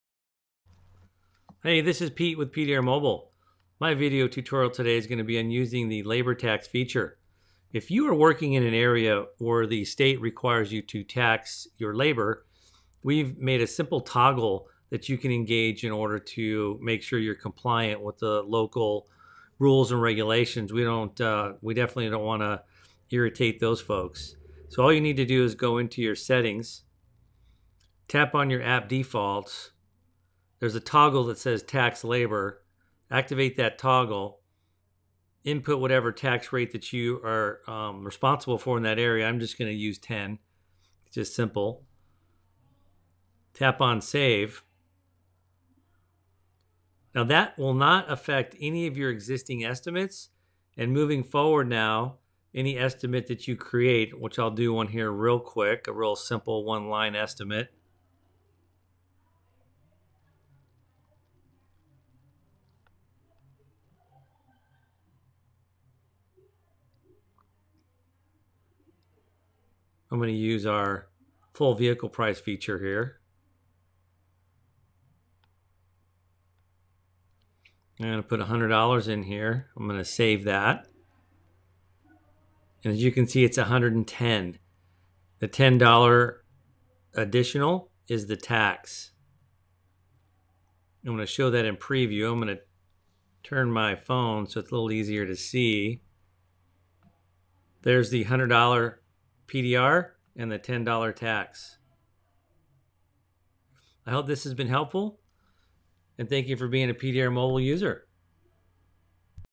Labor Tax Feature
The PDR Mobile estimating app allows you to tax labor and parts through a simple app default setting.